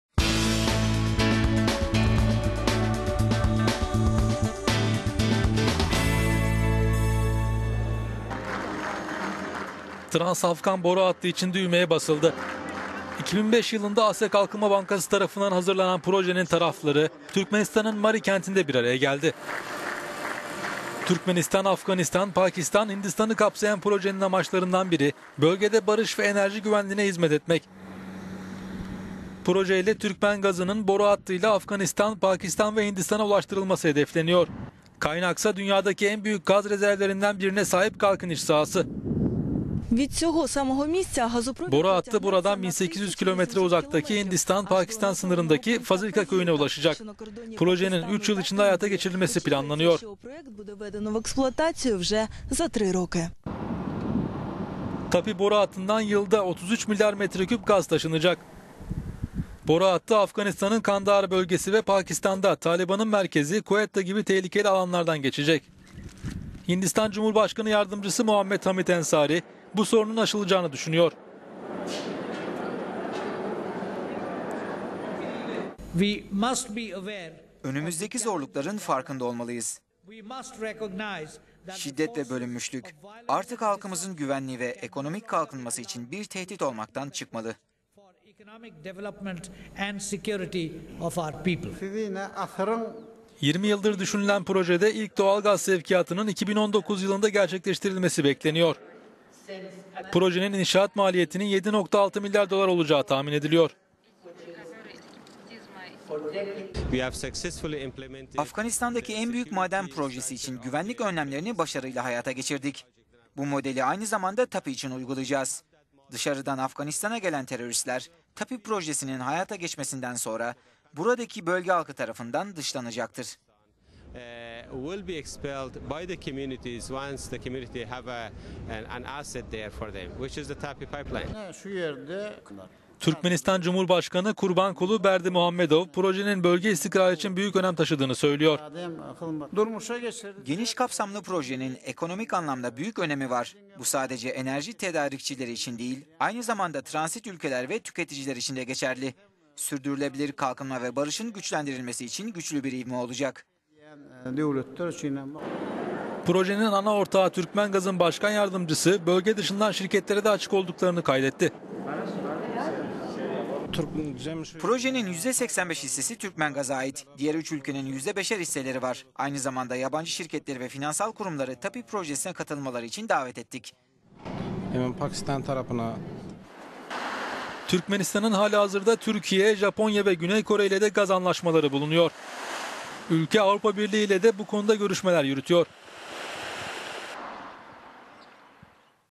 Trans Afgan Boru Hattı için düğmeye basıldı - target
Trans Afgan gaz boru hattının (TAPI) inşaatına başlandı. 2005 yılında Asya Kalkınma Bankası tarafından hazırlanan projenin tarafları Türkmenistan'ın Mary kentinde bir araya geldi.

Türkmenistan, Afganistan, Pakistan, Hindistan'ı  kapsayan projenin amaçlarından biri bölgede barış ve enerji güvenliğine hizmet etmek.

Projeyle Türkmen gazının Hindistan'a kadar ulaştırılması hedefleniyor. Kaynak ise dünyadaki en büyük gaz rezervlerinden birine sahip Galkynysh sahası.